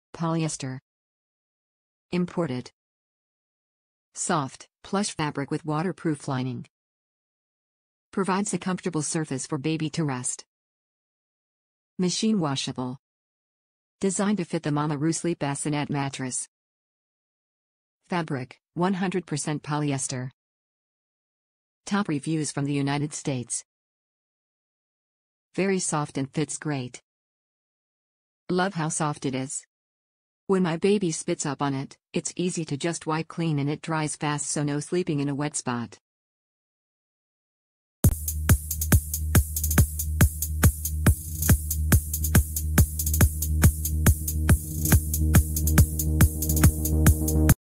4moms mamaRoo Sleep Waterproof Bassinet Sheet - Beads, Grey
4moms mamaRoo Sleep Waterproof Bassinet Sheet - Beads, Grey

Polyester
Imported
Soft, plush fabric with Waterproof lining
Provides a comfortable surface for baby to rest
Machine Washable
Designed to fit the mamaRoo sleep bassinet Mattress
Fabric: 100% polyester

Very soft and fits great.
Love how soft it is. When my baby spits up on it, it's easy to just wipe clean and it dries fast so no sleeping in a wet spot.